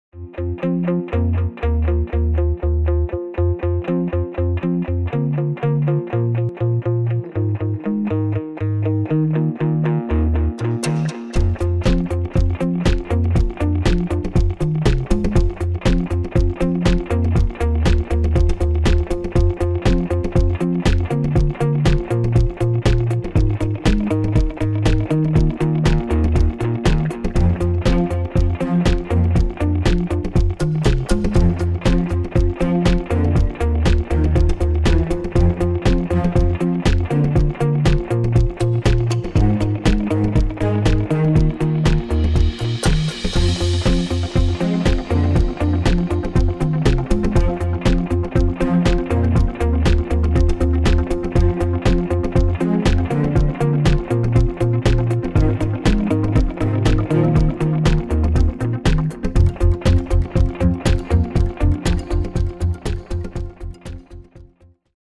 JBL CLIP wireless speaker unboxing
We are unboxing reddot award winner 2014 from company JBL - CLIP. This is rechargeable portable wireless speaker, available in different colors and with practical carabiner so you can easily clip it to your bag and play music wherever you go.